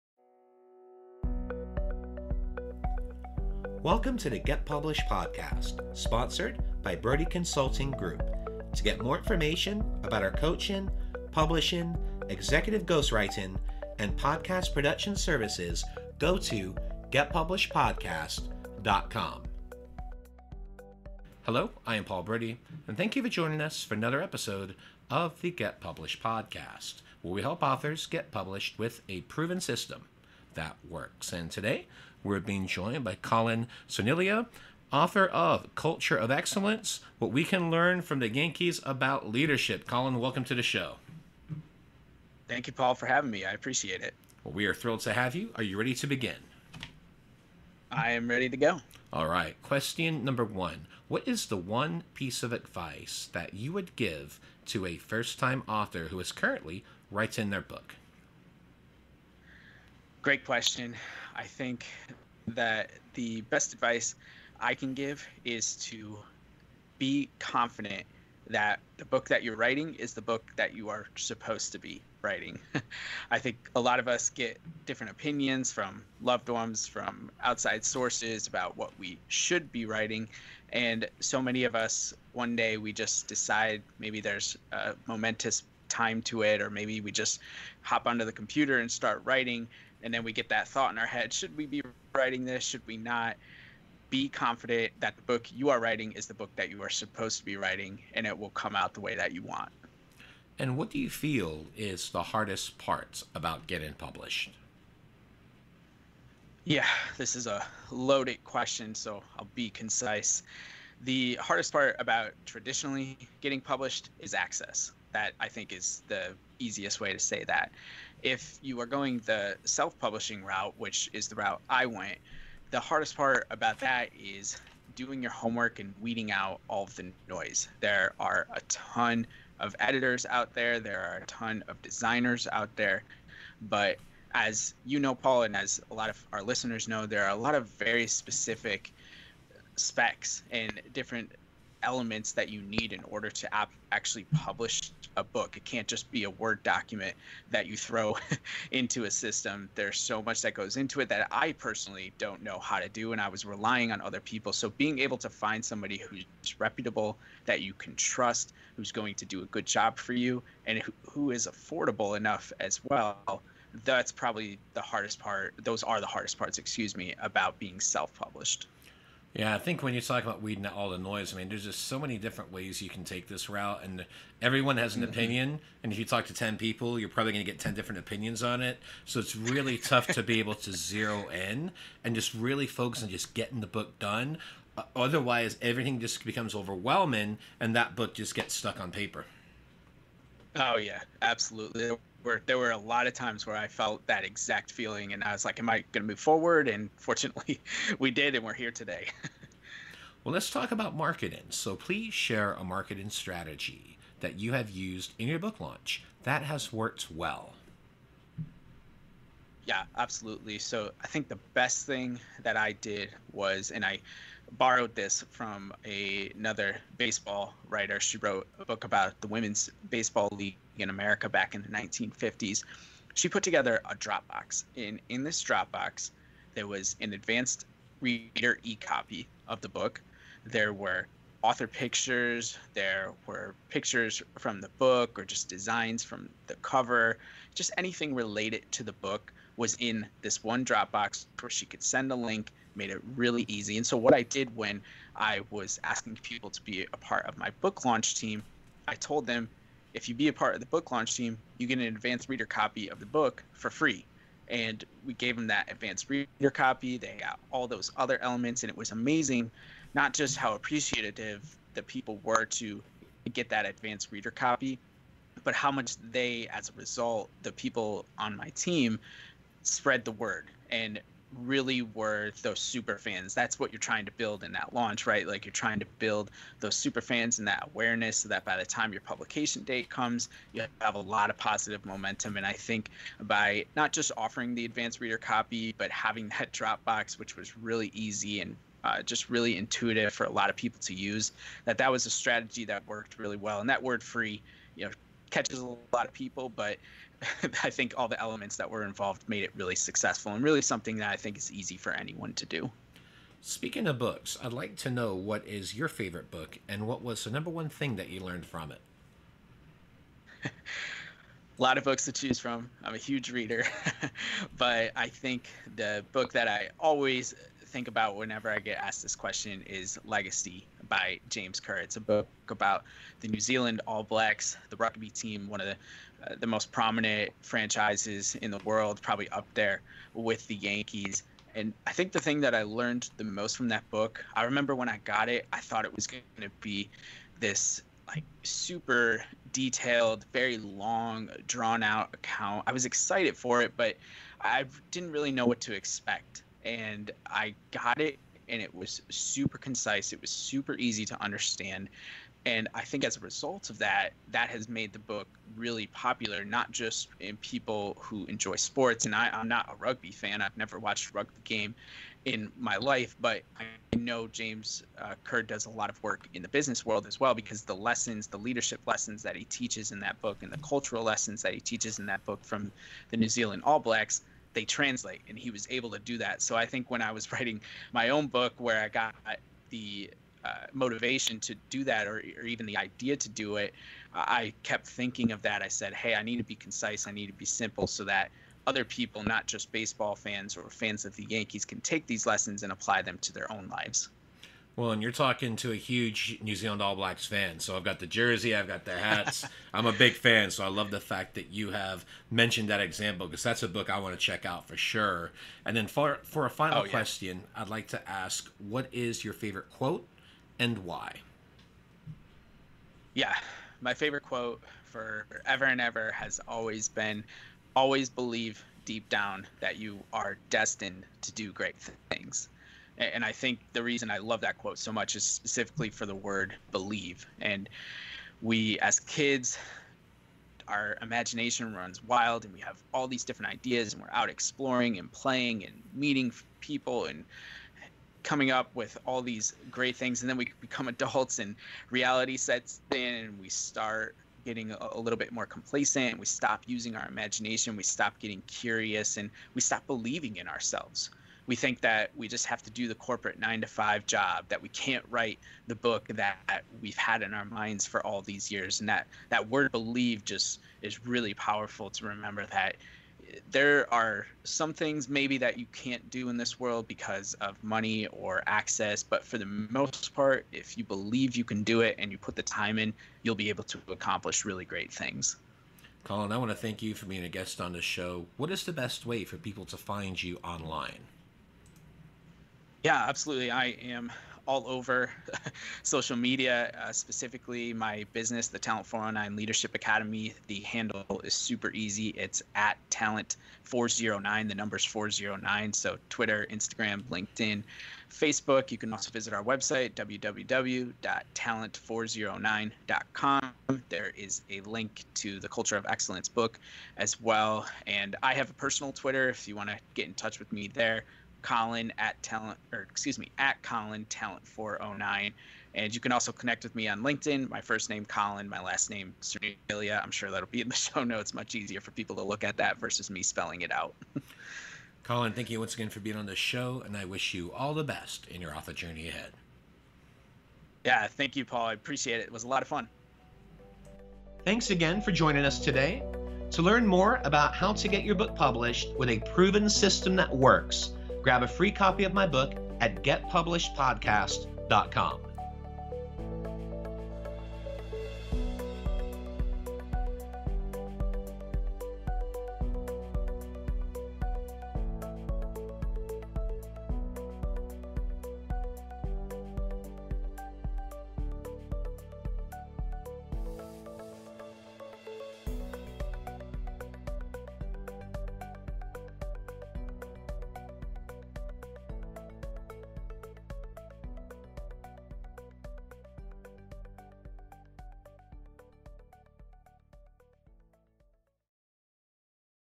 Colin Cerniglia - Using Dropbox for Book Promo Material
In episode 628 of the Get Published Podcast, Host and 15-Time Bestselling Author Paul G. Brodie talks with Colin Cerniglia about how to use Dropbox for book promo material for book marketing.

Find out more about how we can help you Share Your Story